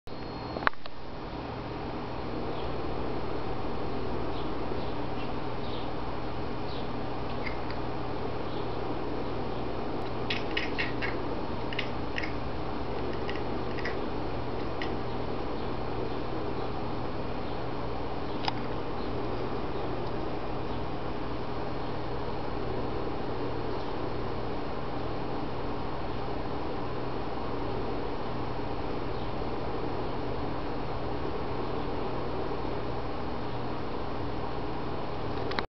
Shara Talking to the Birds
A quick moment of my cat talking to the birds outside.